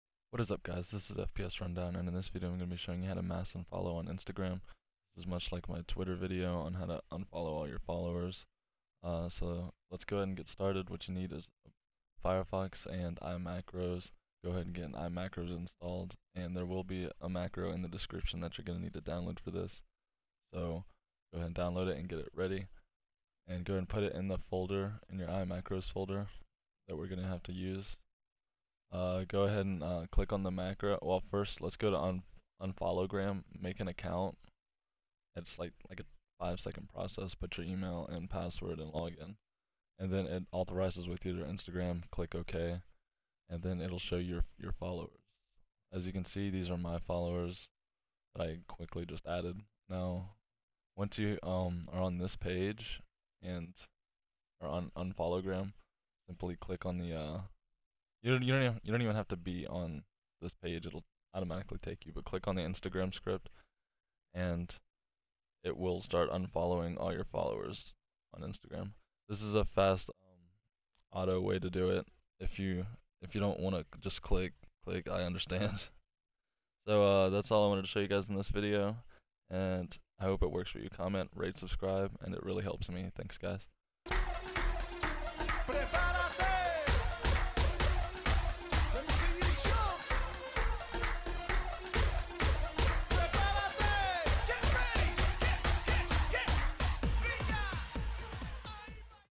★How To Mass Unfollow on Instagram!★
In this video I will be showing you a way where you can quickly unfollow all of your instagram followers without having to go and click them 1 by 1.